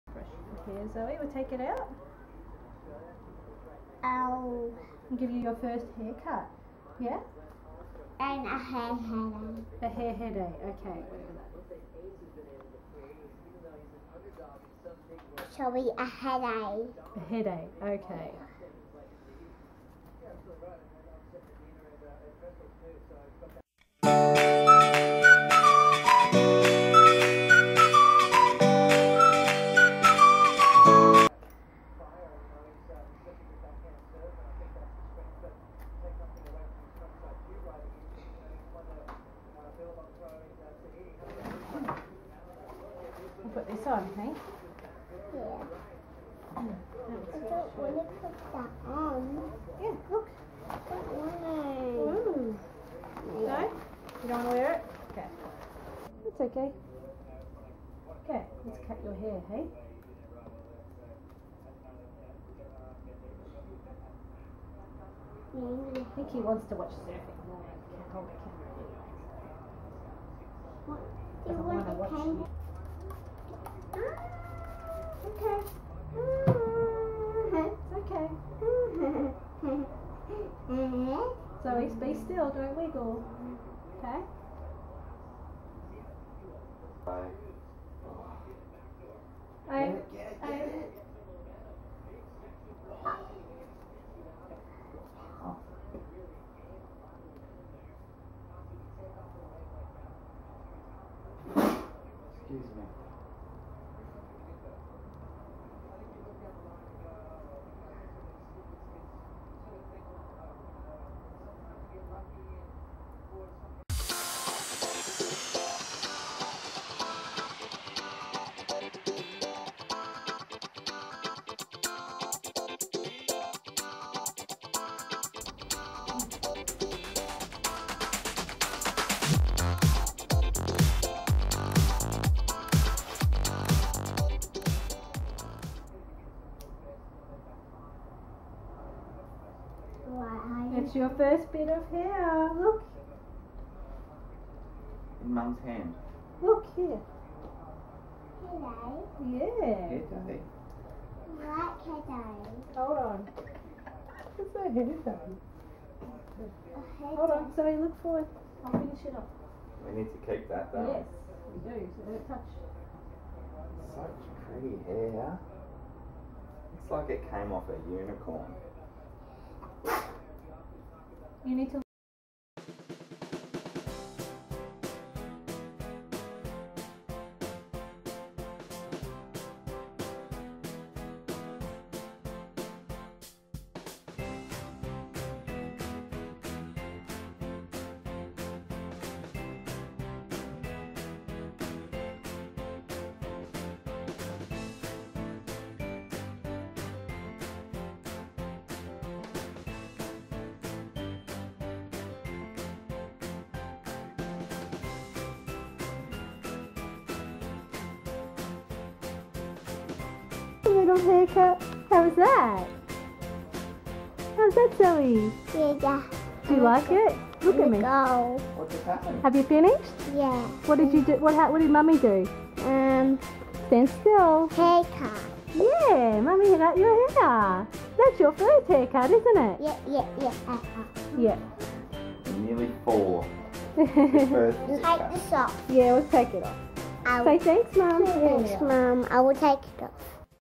Toddlers First Hair Cut! Zoe 3yrs Old - Dec 2014
Our 3 year old toddlers very first hair cut! I remember seeing Ellie Mecham from Ellie And Jared cut their sons hair recently and it reminding me it was time to cut Zoe's!!..Watch more to see her reaction from mommy / mummy cutting her hair and more links below

Hi I'm Jen, an Australian mum of 2 (Zoe aged 3 and Alexandra 1 yrs old), sharing our lives and Australia with the world.